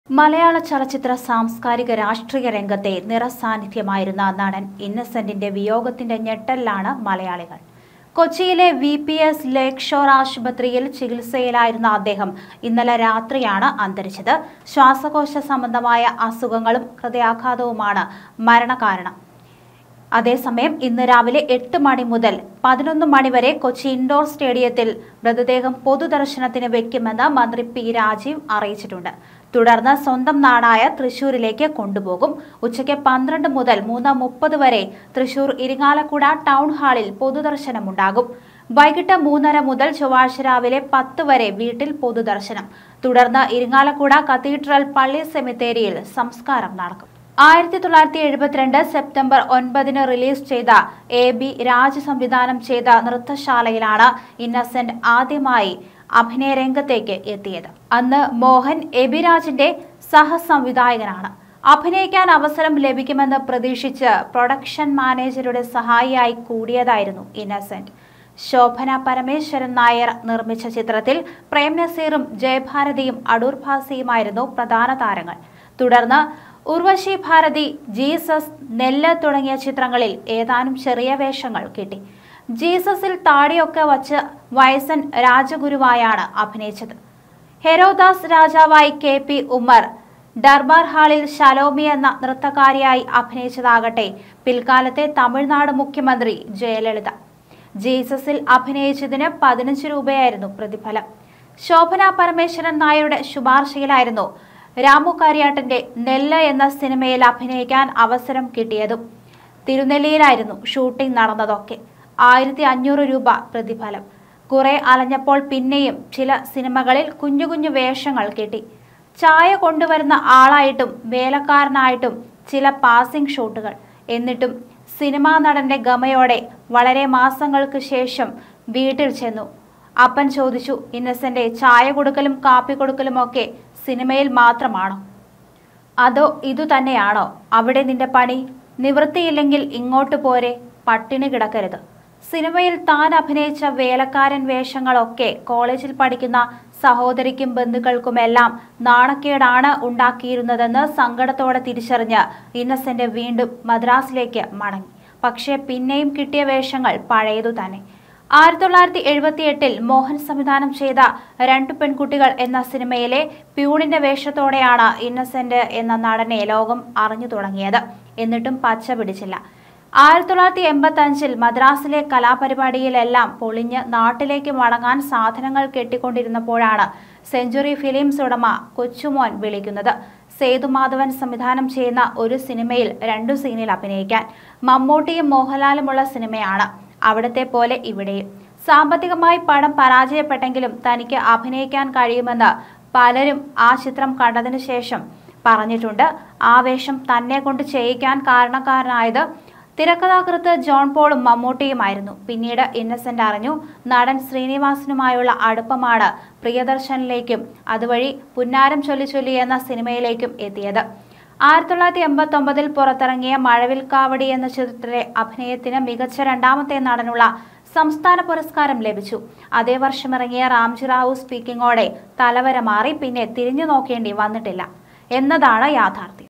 ഇന്നസെന്റിന് ആദ്യം ലഭിച്ച പ്രതിഫലം 15 രൂപ l innocent
മലയാള ചലച്ചിത്ര സാംസ്‌കാരിക രാഷ്ട്രീയ രംഗത്തെ നിറ സാന്നിധ്യമായിരുന്ന നടന്‍ ഇന്നസെന്റിന്റെ വിയോഗത്തിന്റെ ഞെട്ടലിലാണ് മലയാളികള്‍...1972 സെപ്റ്റംബര്‍ 9നു റിലീസ് ചെയ്ത, എ.ബി.രാജ് സംവിധാനം ചെയ്ത 'നൃത്തശാല'യിലാണ് ഇന്നസന്റ് ആദ്യമായി തിരശ്ശീലയിലെത്തിയത്.ഹെരോദാസ് രാജാവായി കെ.പി.ഉമ്മര്‍, ഡര്‍ബാര്‍ഹാളില്‍ ശലോമി എന്ന നൃത്തക്കാരിയായി അഭിനയിച്ചതാകട്ടെ, പില്‍ക്കാലത്തെ തമിഴ്‌നാടു മുഖ്യമന്ത്രി ജയലളിത! ജീസസില്‍ അഭിനയിച്ചതിന് പതിനഞ്ചു രൂപയായിരുന്നു പ്രതിഫലം.